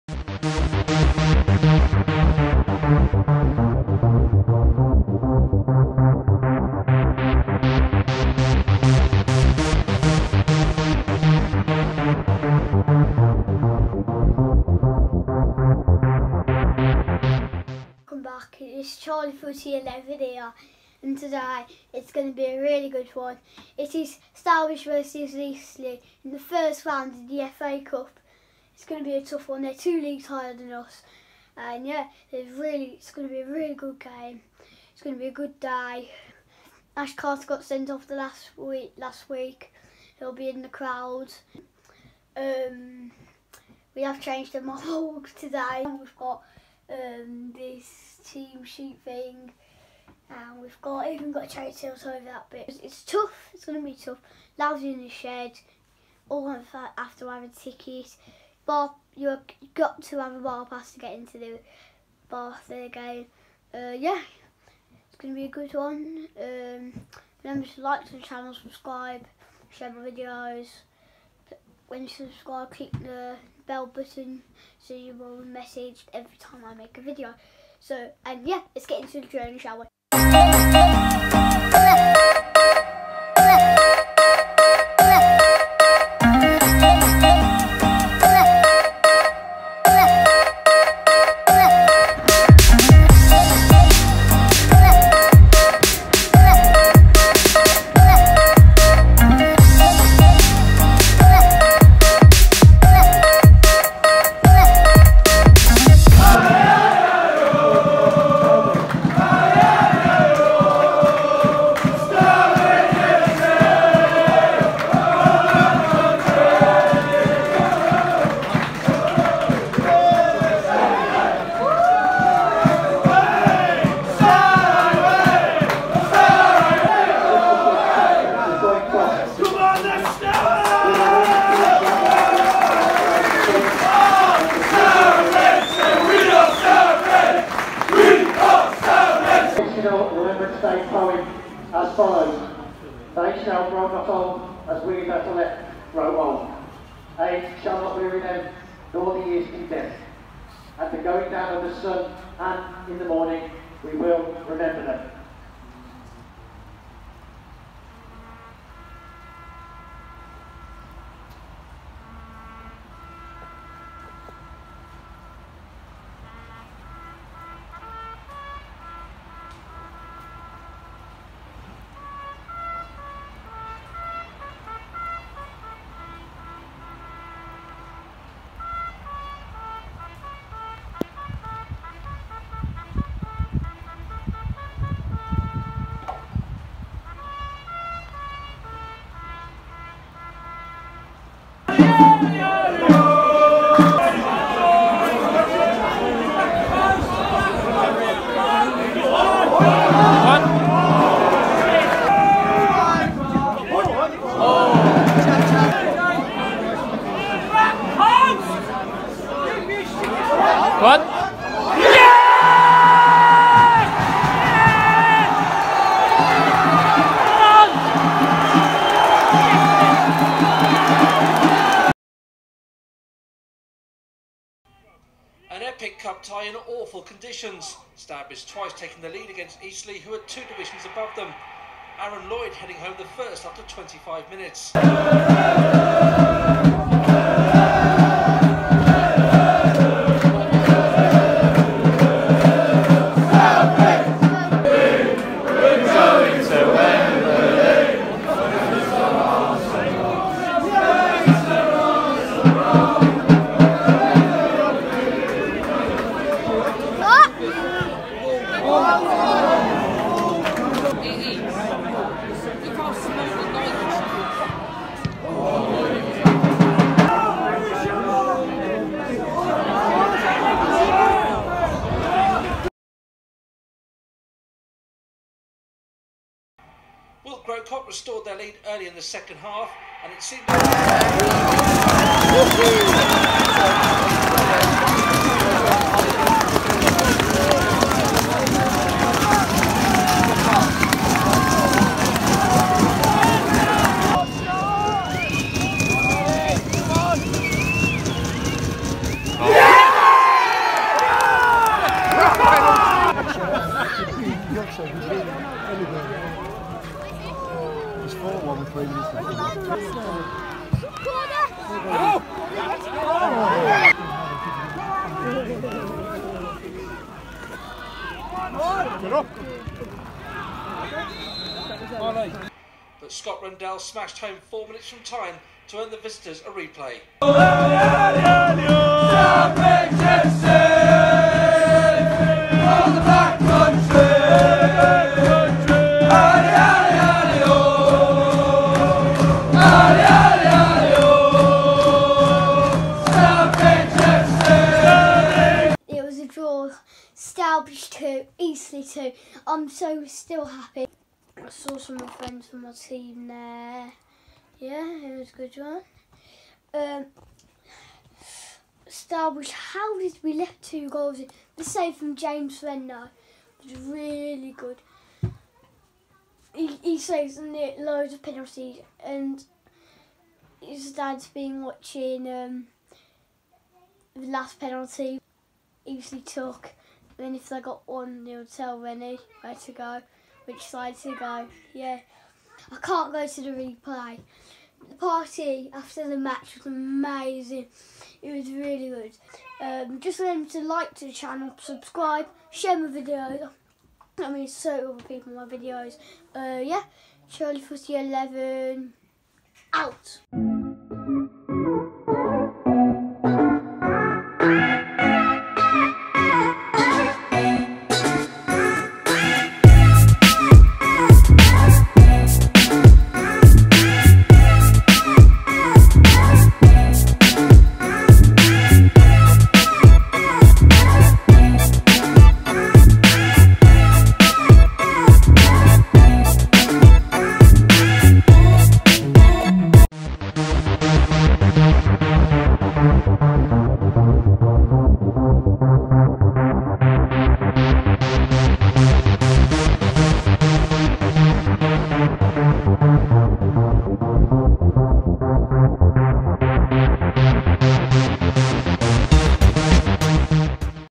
Match Day Vlog #5 Stourbridge FC v Eastleigh - 9/11/19 - FA Cup - Non League CharlieFooty11
Match Day Vlog - Stourbridge FC 2 v Eastleigh FC 2 - FA Cup Round 1

Find and like my pages for up coming video previews!!